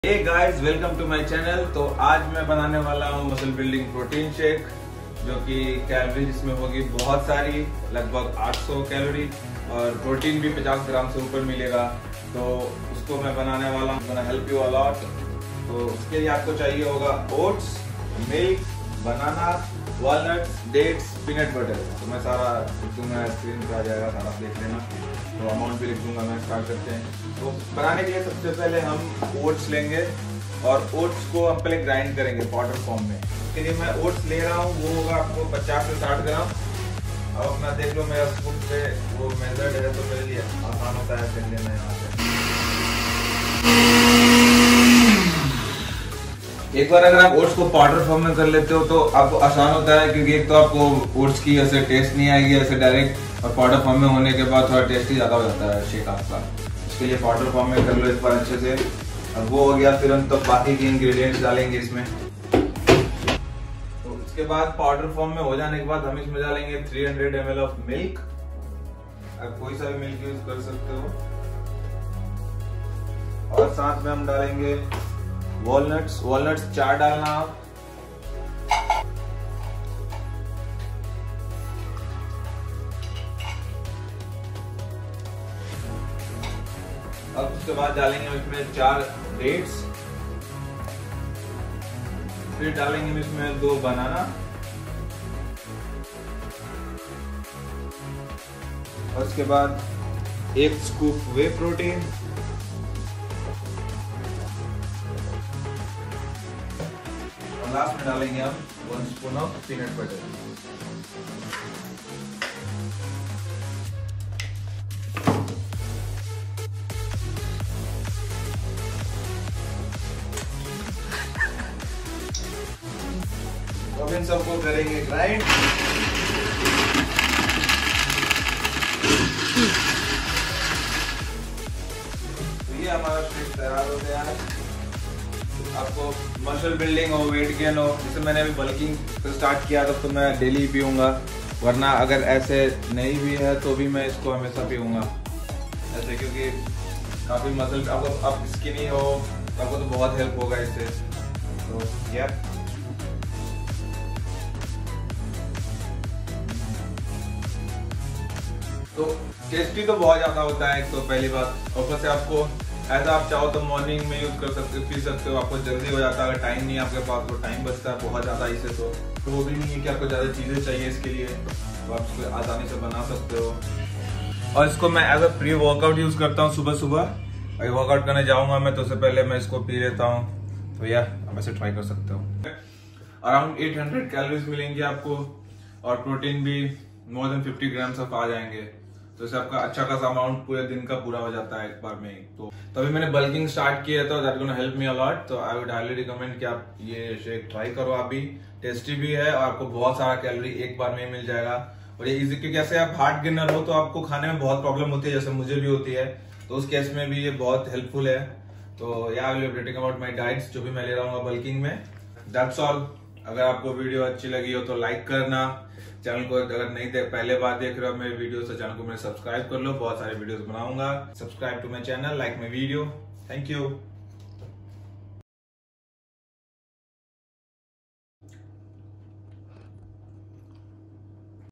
HOMEMADE Mass Gainer/ weight gain Protein Shake for Musclebuilding || Bulking
very easy and effective mass gaining / muscle gaining protein shake receipe you can try at home .
900 calories , 52 grms protein ,
ingridients:
oats (50-60 grms)
milk - 300 ml
banana - 2
peanut butter - 1 tablespoon
walnuts - 4
dates - 4
1 scoop whey protein

share with your skinny friends it will help them to gain weight , muscle and help them in musclebuilding.